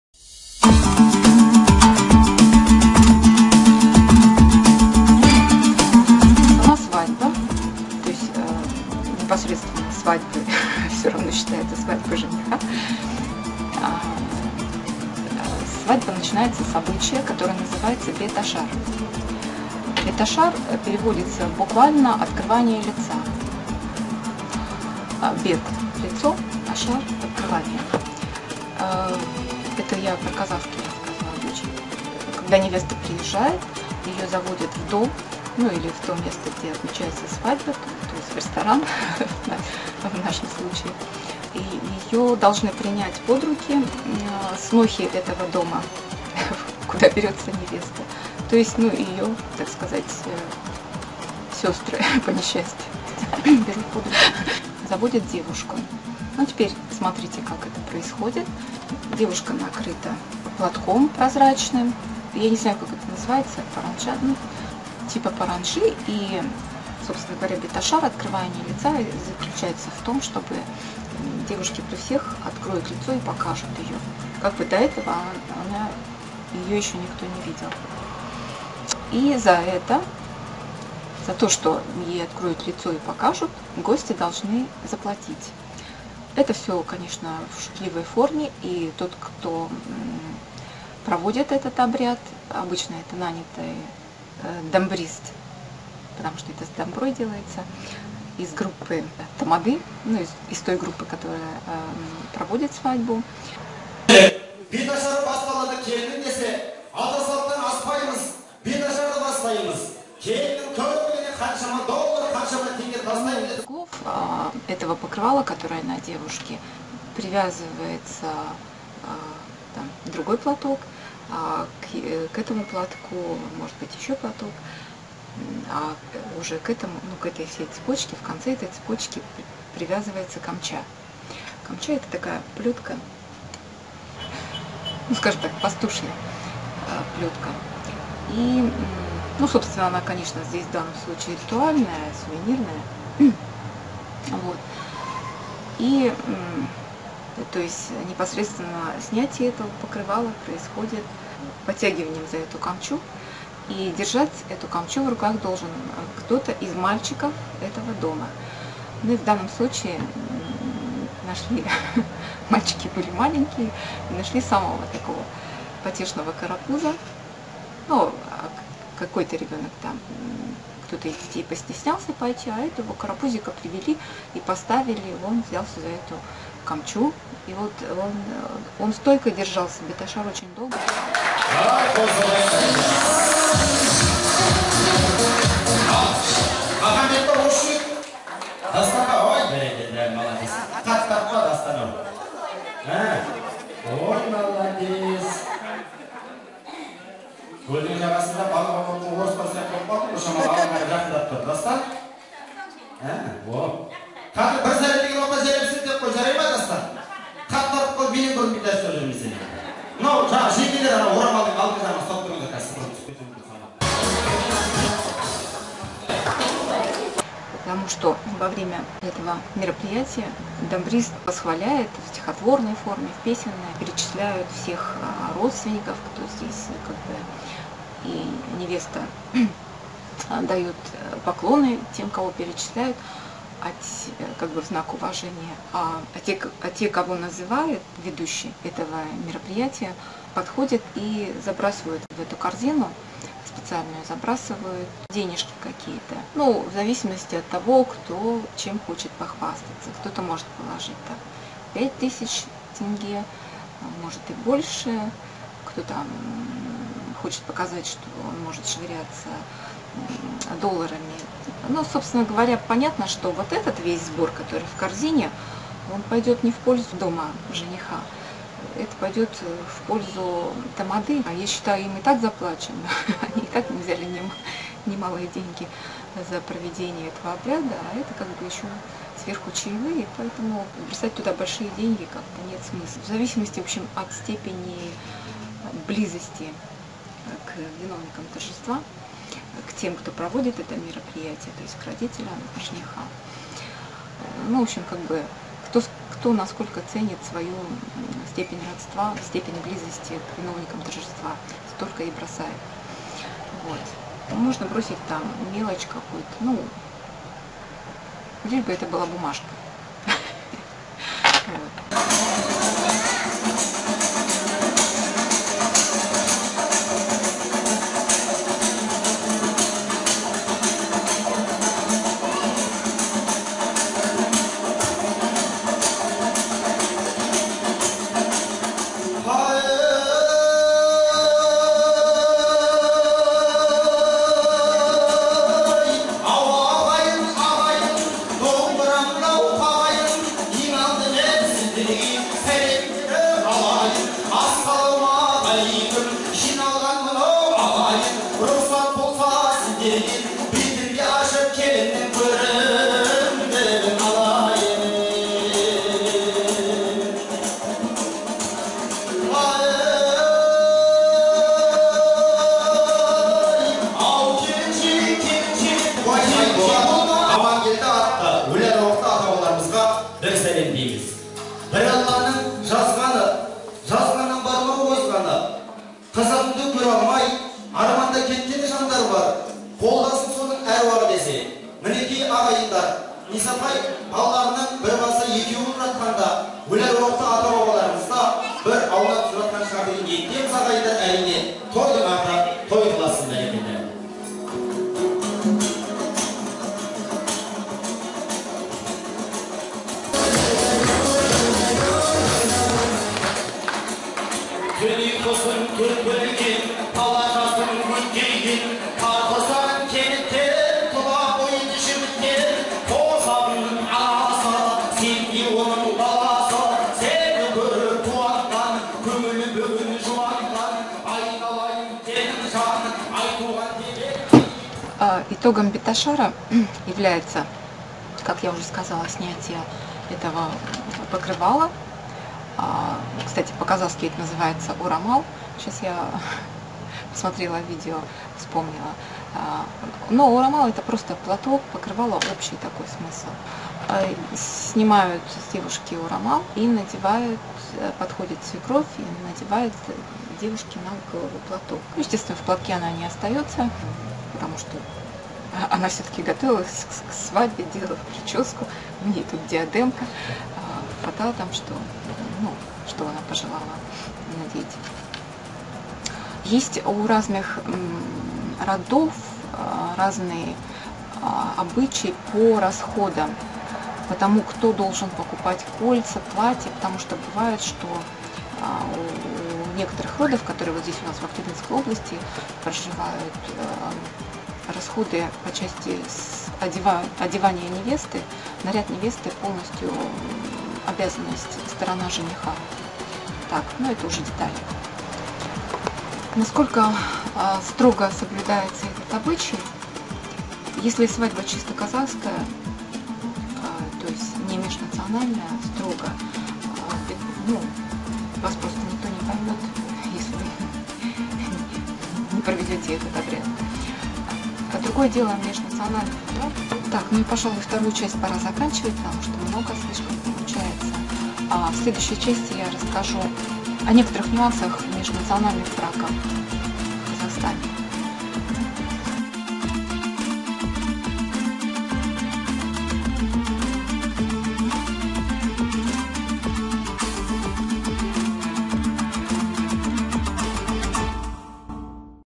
Влог Казахская свадьба моими глазами Беташар показ невесты
Продолжение:
Блюда казахской свадьбы
Похищение невесты - что это за обычай?